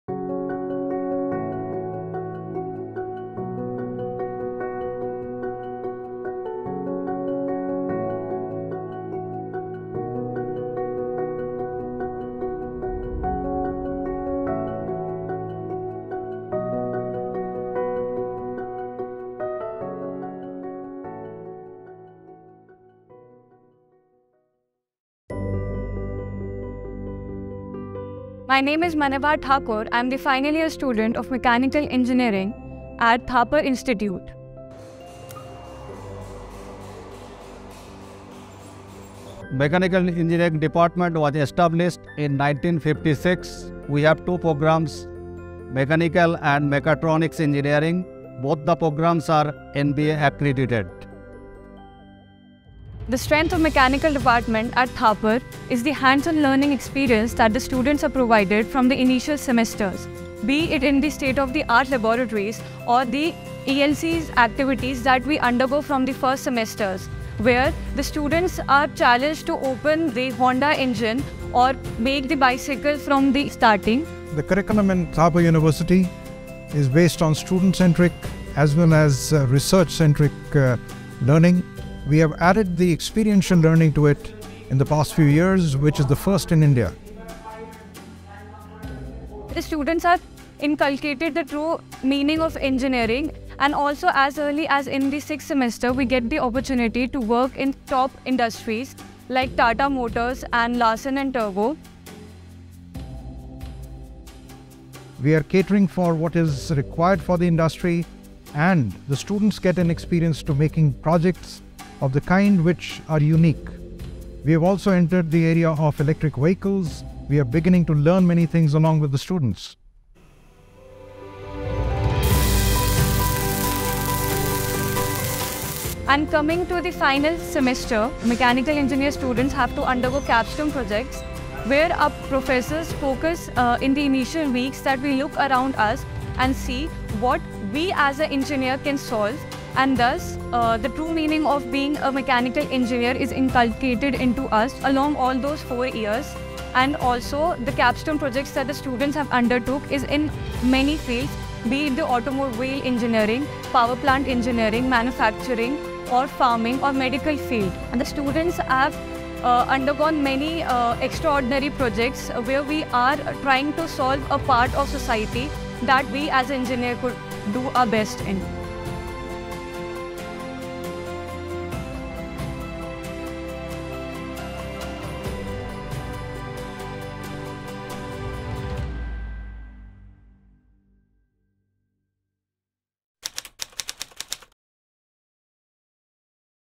Mechanical Engineering at Thapar Institute
Pursuing Mechanical Engineering at Thapar Institute provides an excellent learning experience, exposure to industry, and career opportunities. The combination of excellent faculty, state-of-the-art infrastructure, industry collaboration, global exposure, and strong placement record makes it a great choice for students. Listen to the faculty and mechanical students in this video.